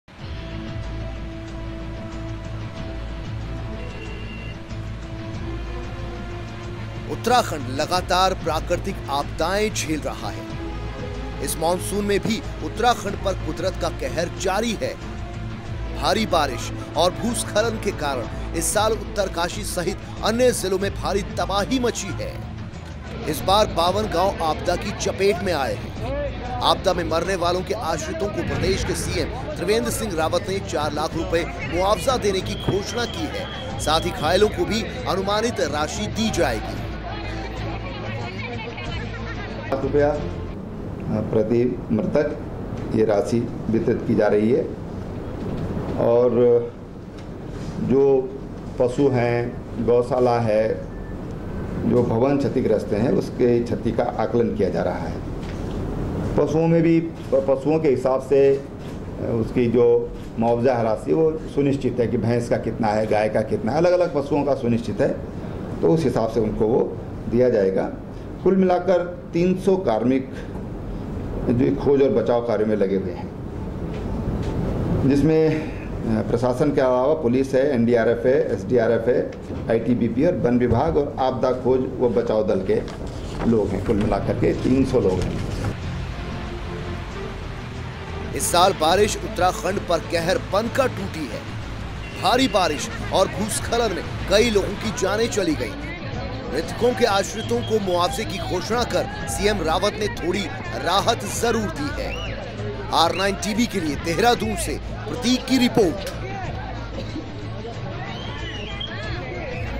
सीएम रावत ने आपदा पीड़ितों को दी राहत
उत्तराखंड आपदा में मरने वालों के आश्रितों के लिए सीएम रावत ने 4 लाख रुपये के मुआवजे की घोषणा की है..साथ ही आपदा में घायल हुए लोगों को भी अनुमानित राशि दी जाएगी..